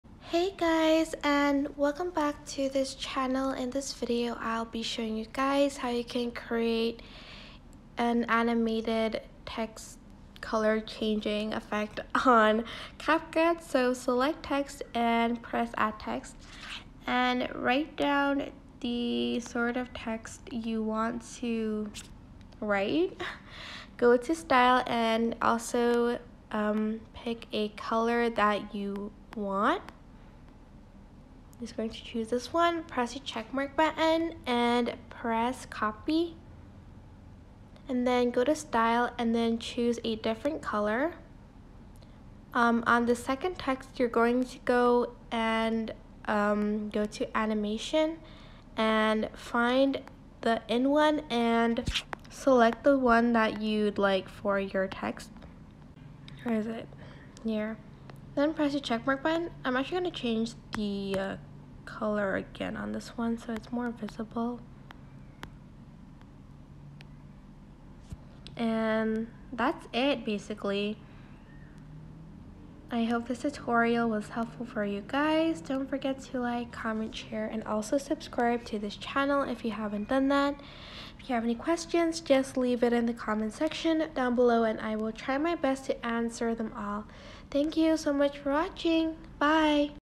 Color Changing Text Animation Tutorial On CapCut
Learn how you can make a color changing text animation on CapCut by watching this tutorial.